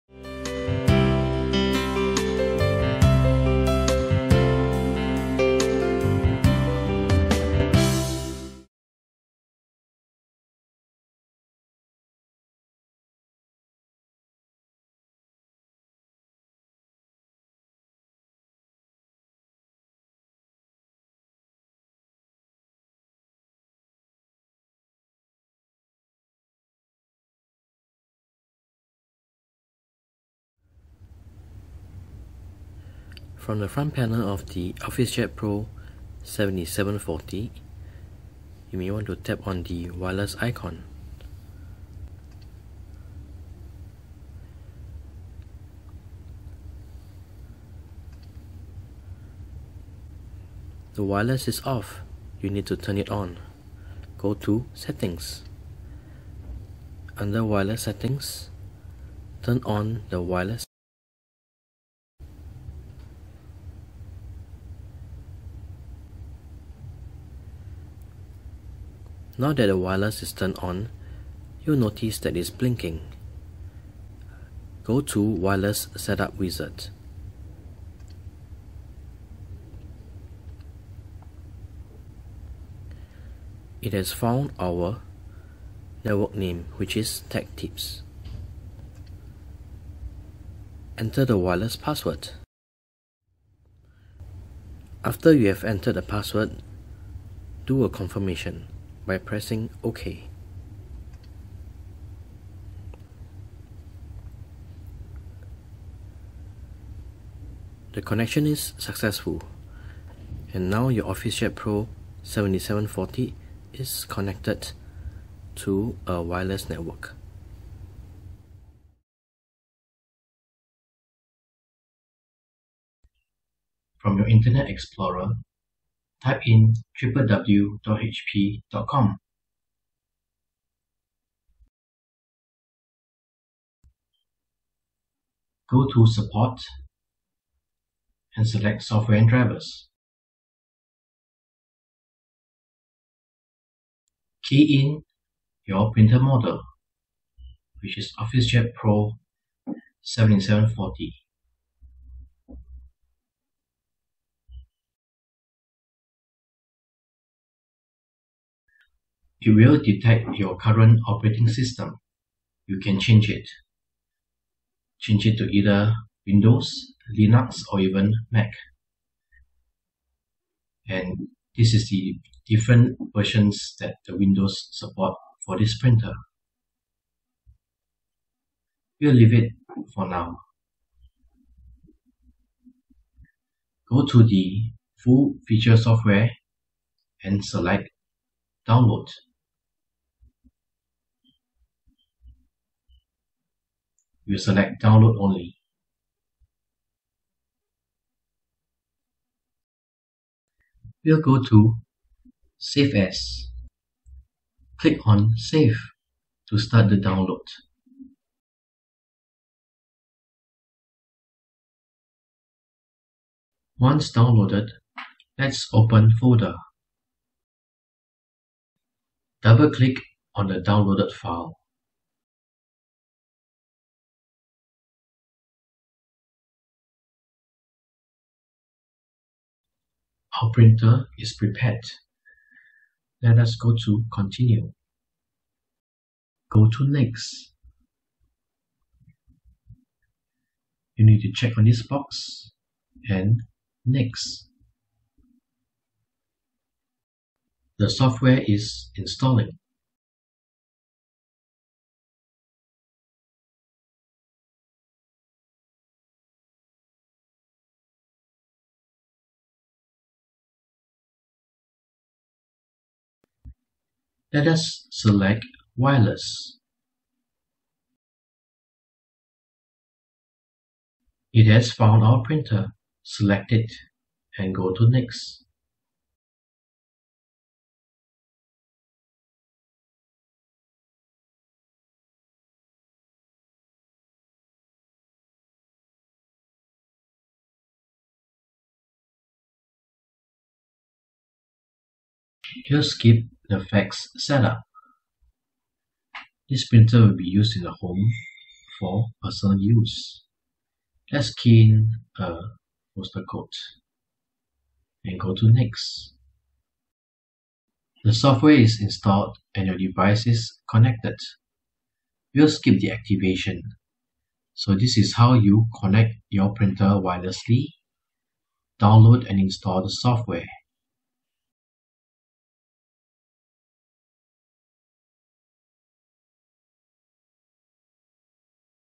HP Officejet Pro 7740 Connect wirelessly, Download & Install Software
This tutorial shows how to Connect wirelessly, Download & Install the software for the HP Officejet Pro 7740 AiO printer.

The HP Officejet Pro 7740  is a printer that offers Print, Copy, Scan & Fax.

It prints, copy & scans in sizes up  to 11x17"; Fax up to 8.5 x 11".

It has a 35-sheet ADF that can scan 2-sided. It also has Auto duplex printing capabilities.